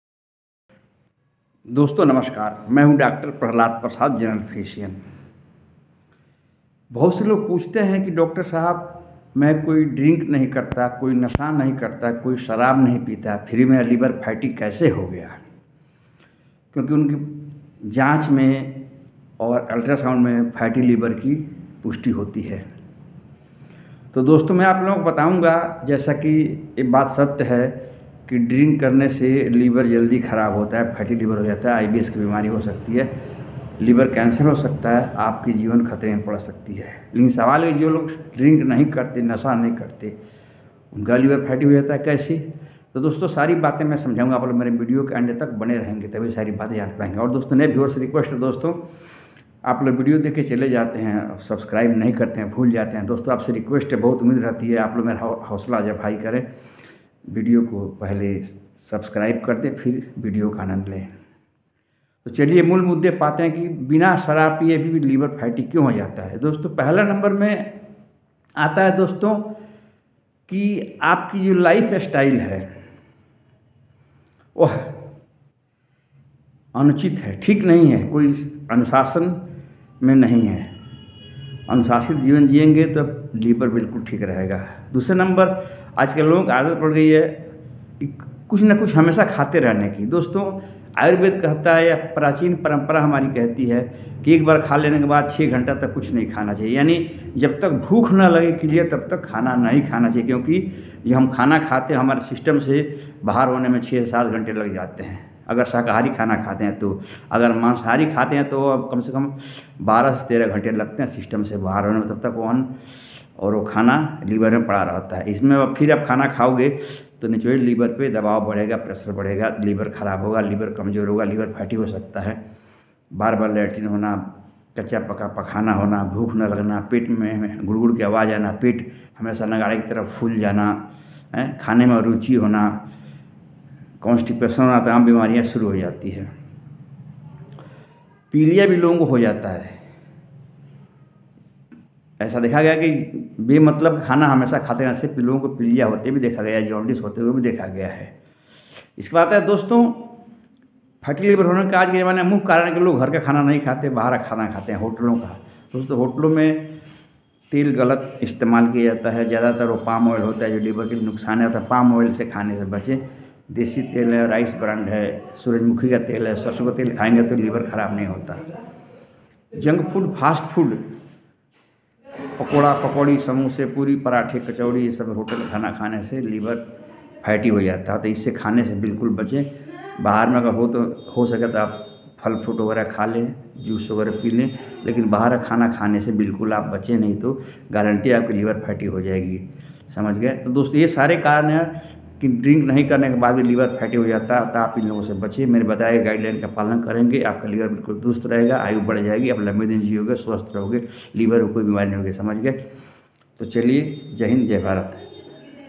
शराब नहीं पीते फिर भी लीवर फैटी ( Fatty liver ) क्यों हो जाता है || V health.
शराब नहीं पीते फिर भी लीवर फैटी ( Liver Fatty ) क्यों हो जाता है।